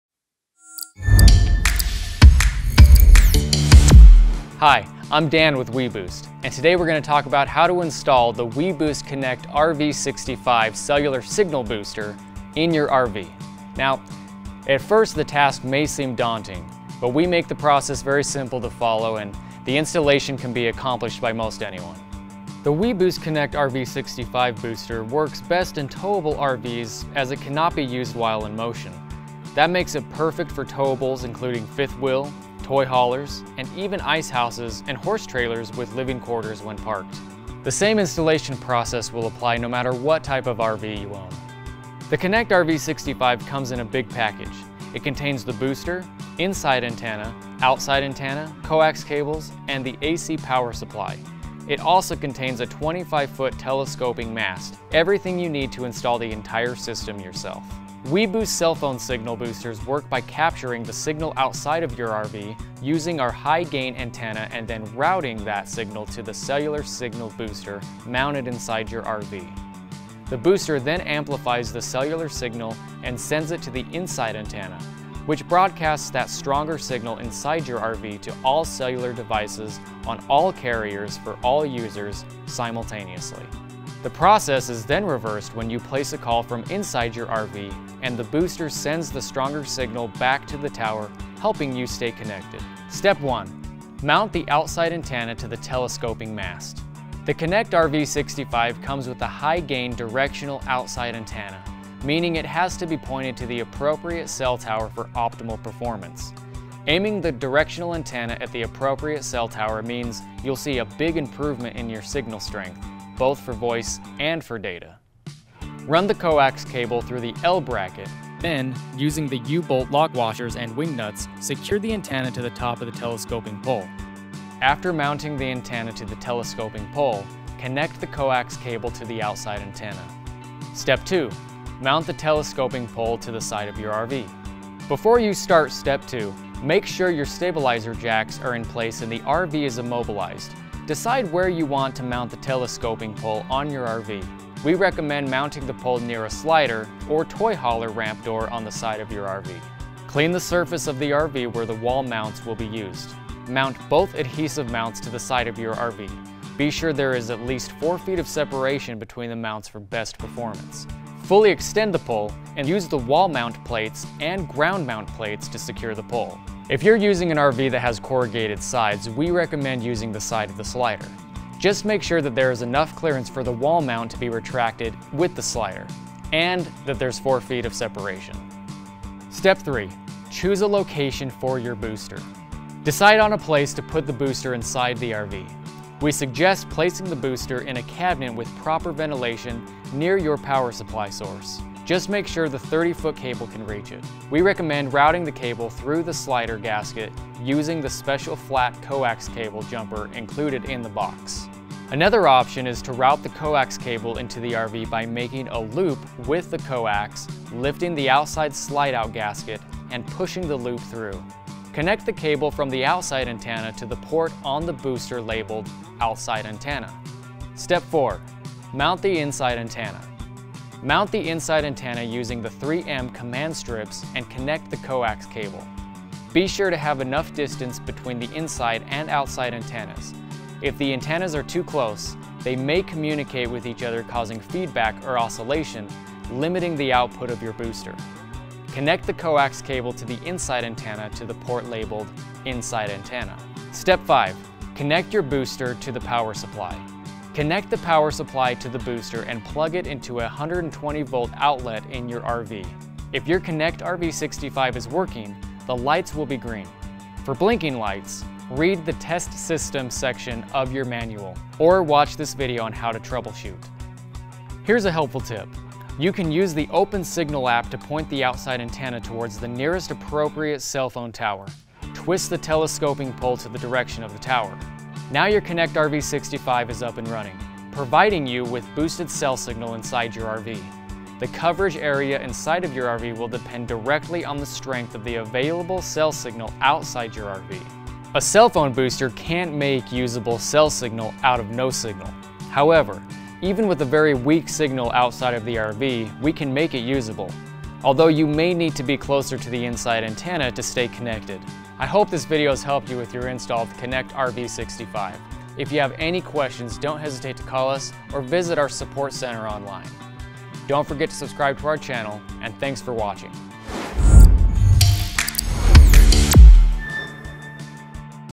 Installing the Connect RV 65, cell signal booster for your RV | weBoost
In this video we walk you through the installation of the weBoost Connect RV 65 cellular signal booster in your RV. This may seem like a challenging project, but Dan breaks it down into simple steps. This install can be done almost anyone.

The Connect RV 65 is a powerful cell signal booster designed specifically for towable RVs and toy haulers. Law enforcement departments and other first responders will find this cell booster useful in applications like mobile command centers and tactical operations centers.

This booster can be used in any kind of RV or trailer, but it works only while the RV is stationary and can't operate while it's in motion. That's because the FCC allows stationary cell boosters to operate with greater gain, or signal boost, than mobile boosters.

So the Connect RV 65 has more gain, up to 65 dB as the name states, than any mobile cellular boosters, which are capped at 50 dB gain by FCC regulations.

That means the Connect RV 65 gives you much greater boost for incoming cells signals from the tower, and for signals you send from your phone or other cellular device back to the tower.

Like we said it's FCC certified, and it works with all U.S. cellular carrier networks, even the smaller regional service providers.

This kit has a telescoping pole (it extends up to 25' tall) which allows the outside directional antenna to collect weak signals from the nearest cell tower. The booster then amplifies those signals and redistributes the amplified signals inside the RV or trailer so all cell devices can connect.

The Connect RV 65 support multiple device connections to multiple carrier networks simultaneously. It boosts voice and data, including 4G LTE, so it's a perfect companion for streaming media hubs at the camp site.

Initial installation takes approximately 30 minutes, with about 10 minutes needed to set up and take down the pole and outside antenna each time the RV is parked.